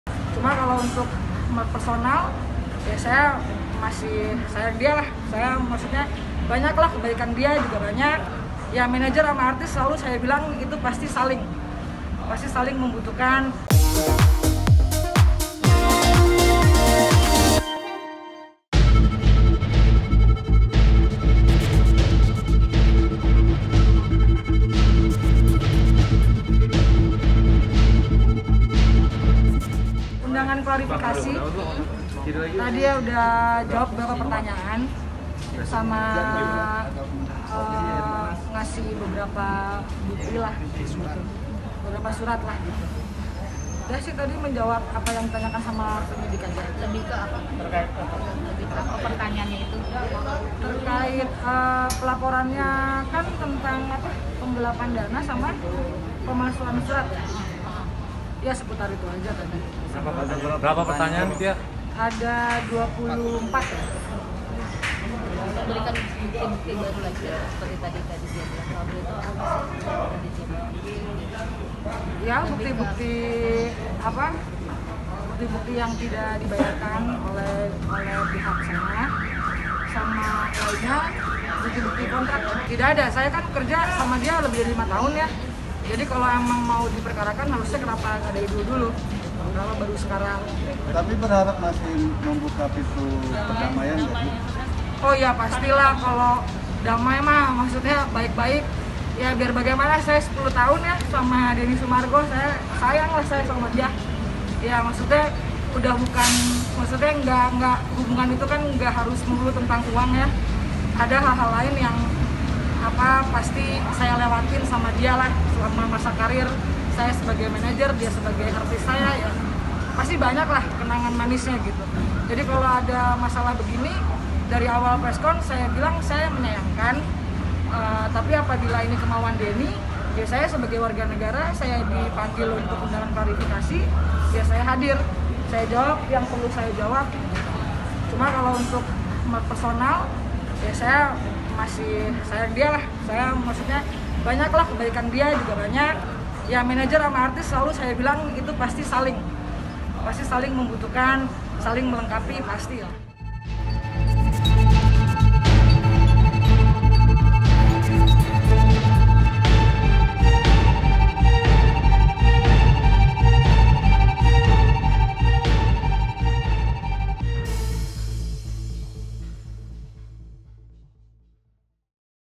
Diperiksa Polisi, Ditya Andrista Masih Sayang Dengan Denny Sumargo
KAPANLAGI.COM - KLovers,
Eks manajer Denny Sumargo, Ditya Andrista, diperiksa pihak kepolisian terkait laporan sang pebasket. Ditya ditemani kuasa hukumnya memberikan klarifikasi dan  menyerahkan bukti baru terkait laporan tersebut. Ia mengaku membuka pintu damai karena masih sayang dengan Densu.